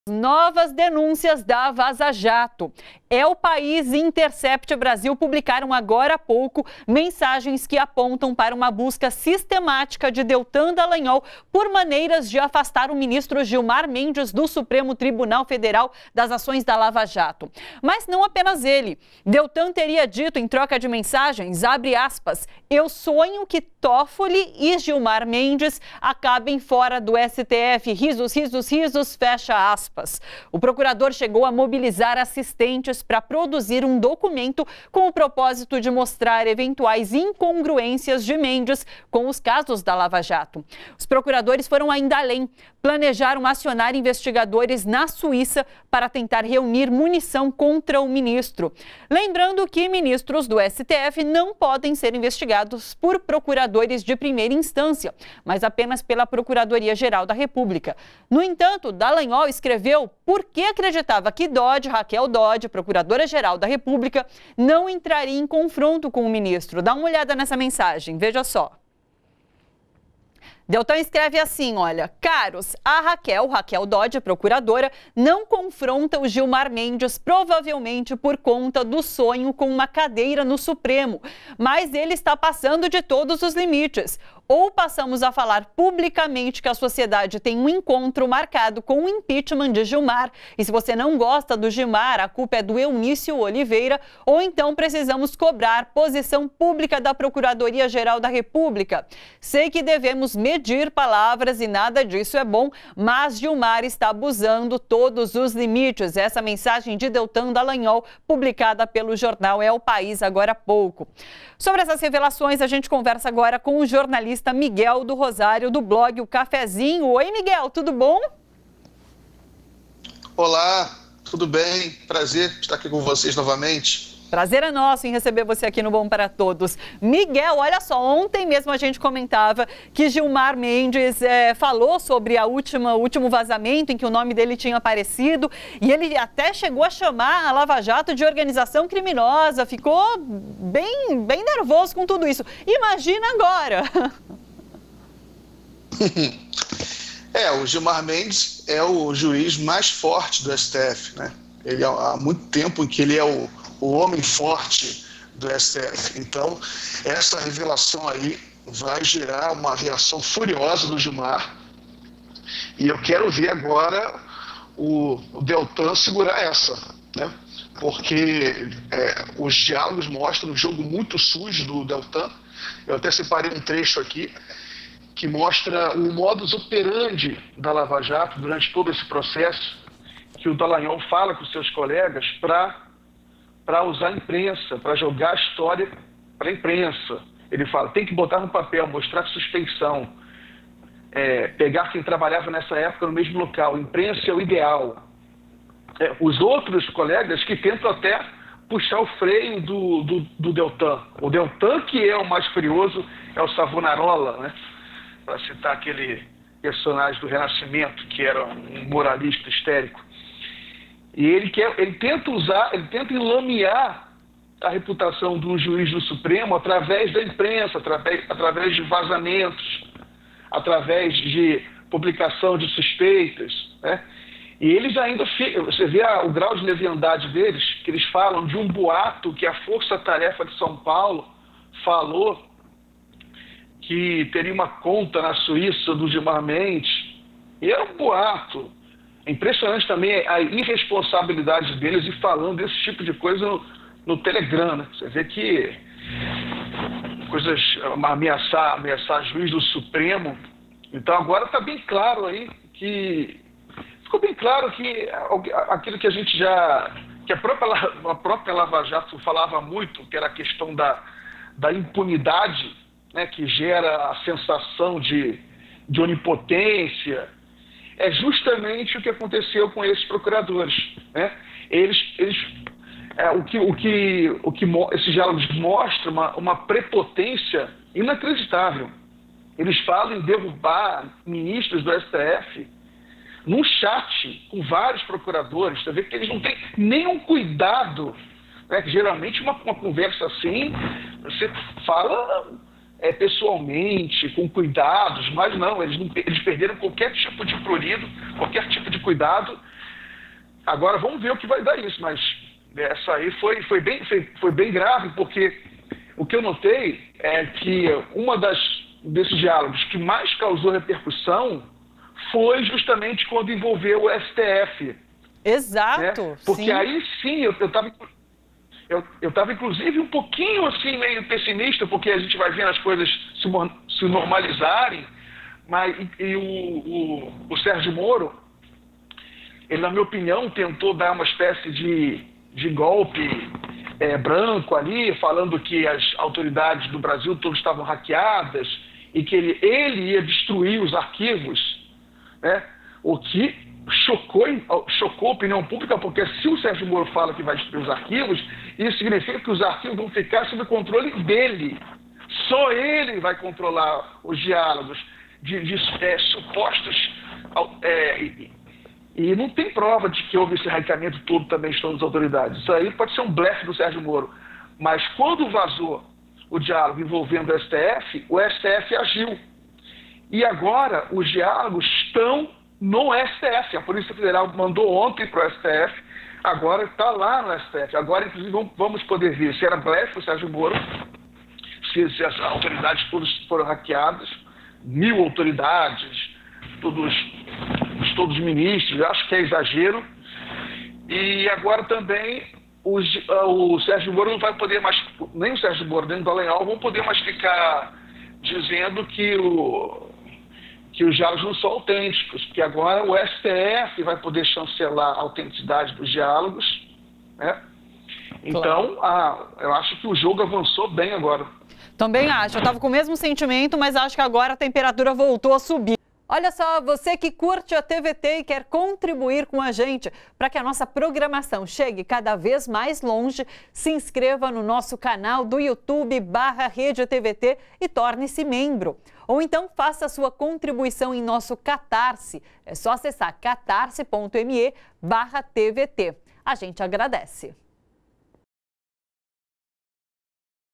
Dallagnol quis derrubar juízes do STF
Novas denúncias da Vaza-Jato: El País e The Intercept Brasil publicam mensagens que apontam para uma busca sistemática de Deltan Dallagnol por maneiras de afastar o ministro Gilmar Mendes do Supremo Tribunal Federal das ações da Lava Jato. Quem comenta é Miguel do Rosário, do blog O Cafézinho.